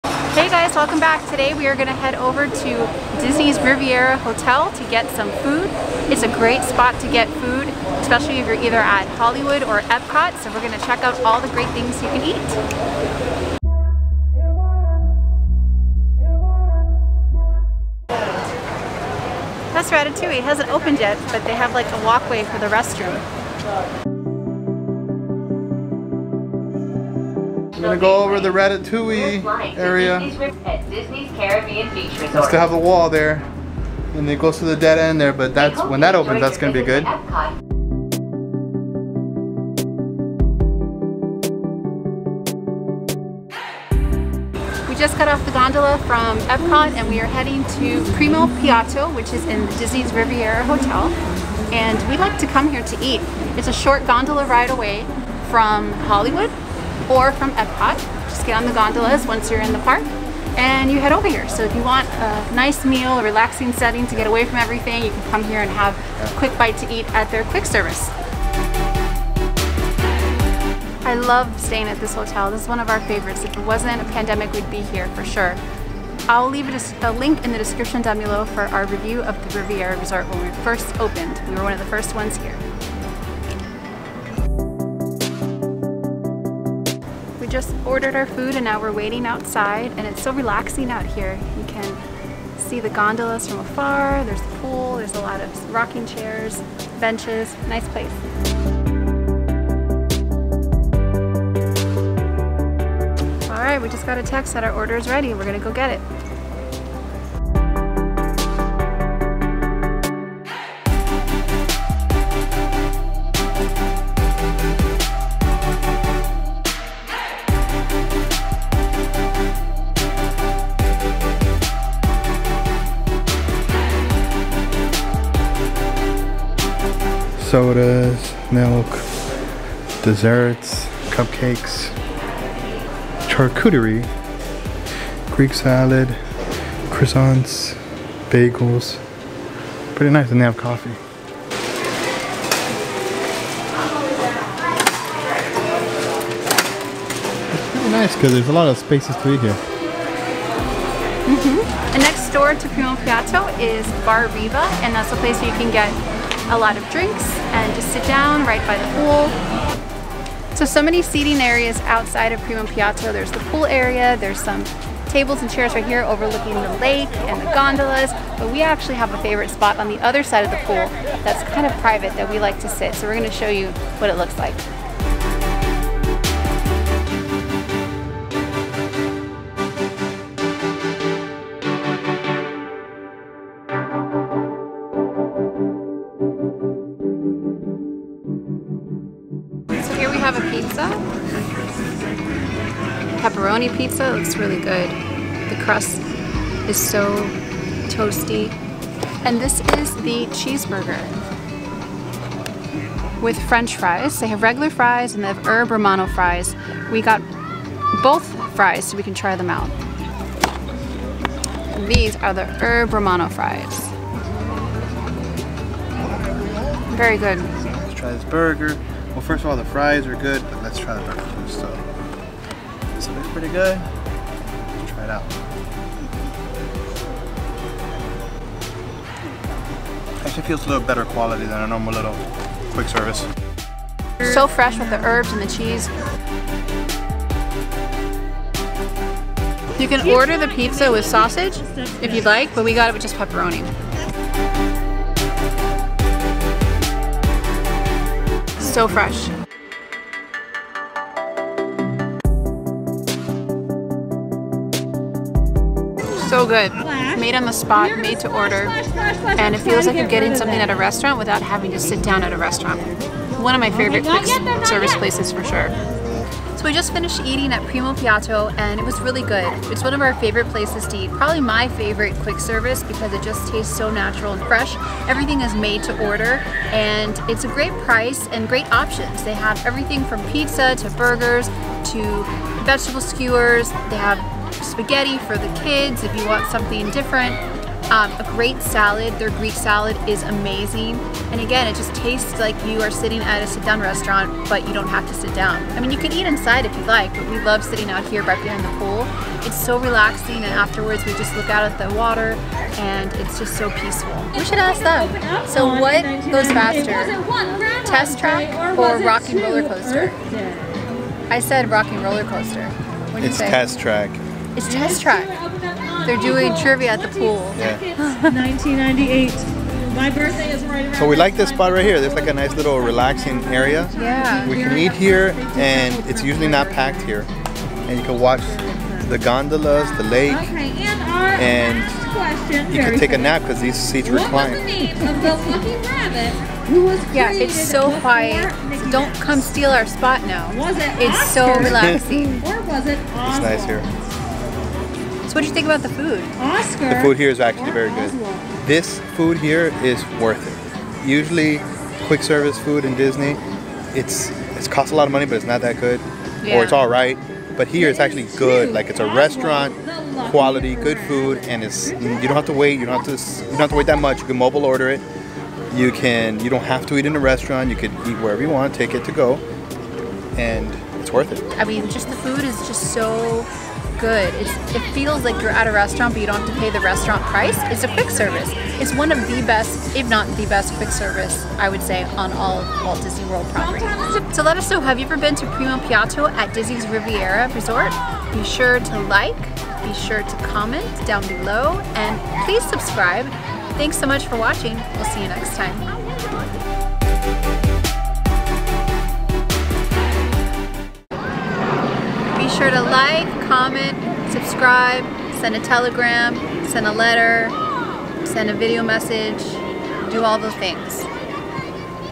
Primo Piatto at The Riviera Resort is Disney's Best Quick Service Restaurant!
Primo Piatto at Disney's Riviera Resort is the best quick service restaurant on property. Restaurant quality food at quick service prices.